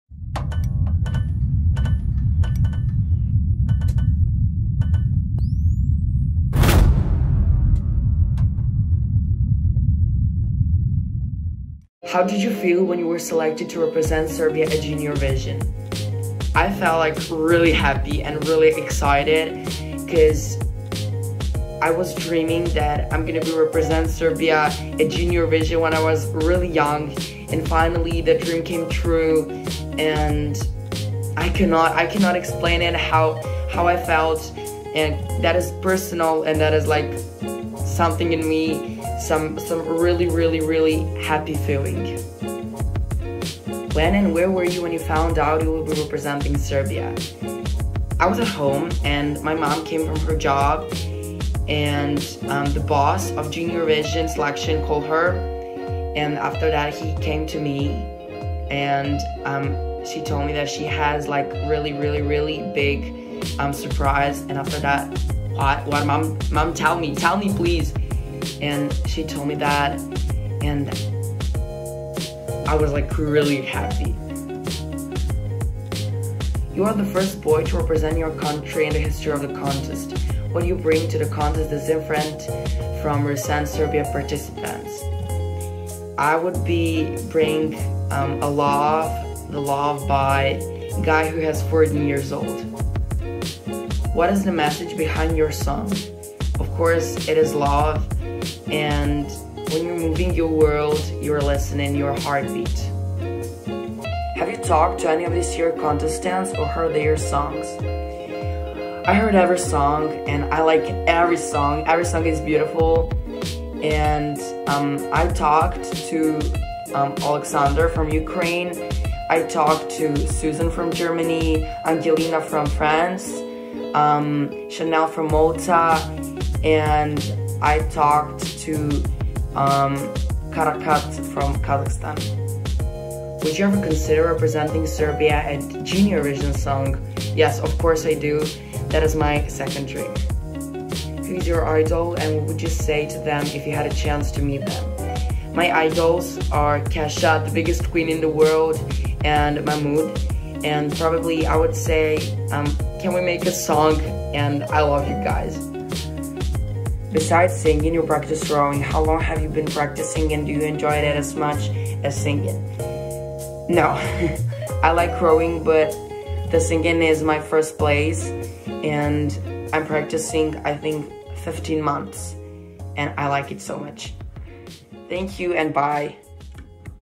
Serbia: An Interview With Petar Aničić
Ahead of this year's Junior Eurovision Song Contest, we talk to Petar Aničić, Serbia's Junior Eurovision 2020 entrant. Thank you Petar for the interview.